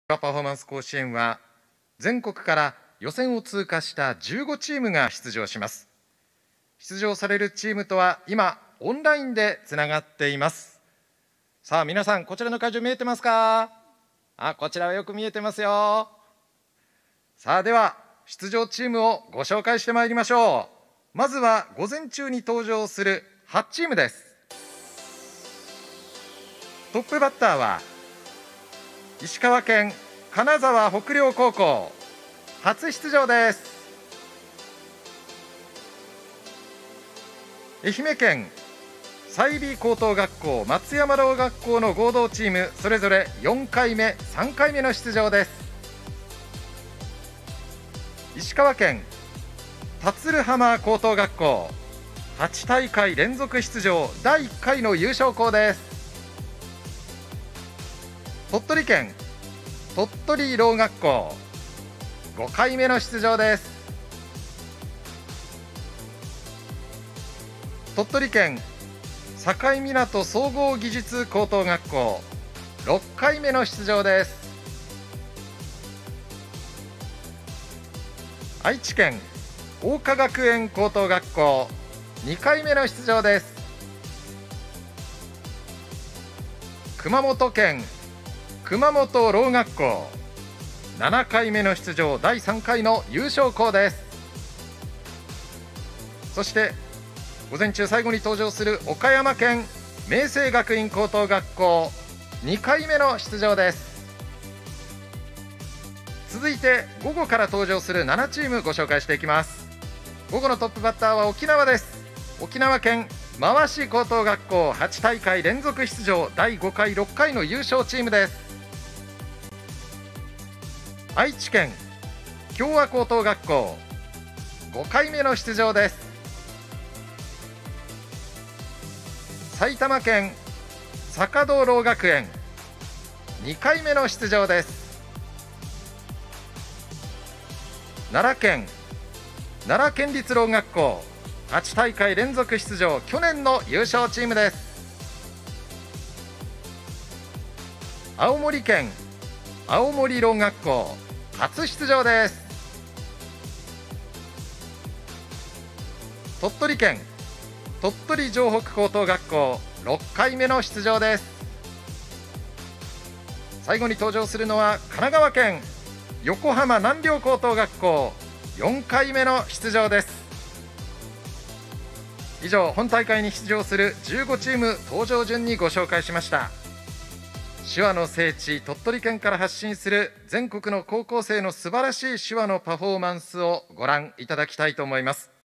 2021.10.03 第8回 全国高校生手話パフォーマンス甲子園 予選通過 本選参加校紹介(コロナウイルス感染防止のため、各校の生徒はリモート出演しています。)
ディスプレイには、鳥取県外の12校、鳥取県内の3校、合計15校が、映っています。